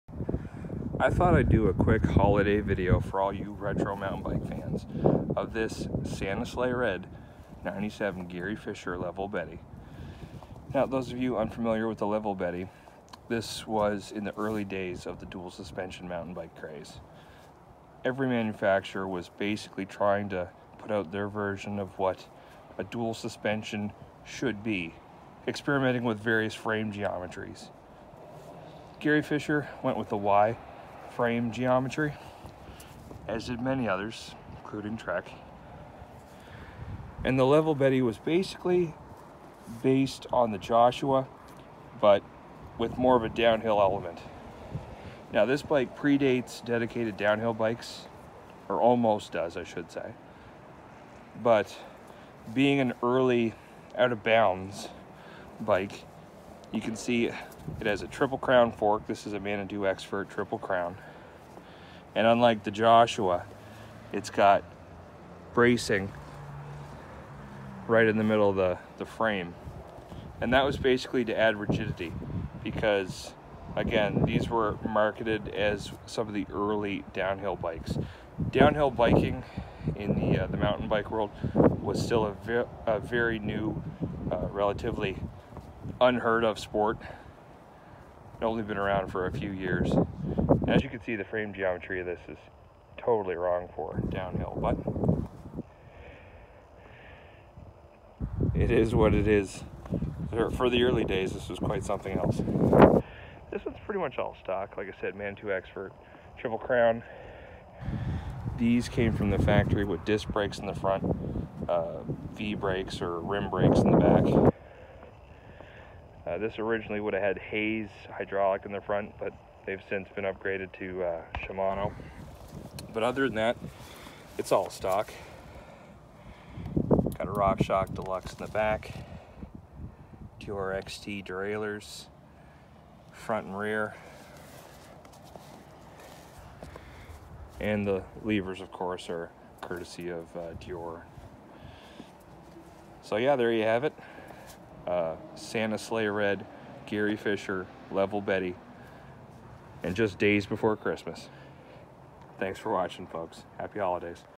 Holiday Retro mountain bike review - Gary Fisher Level Betty (1997)
Level Betty all mountain / down hill bike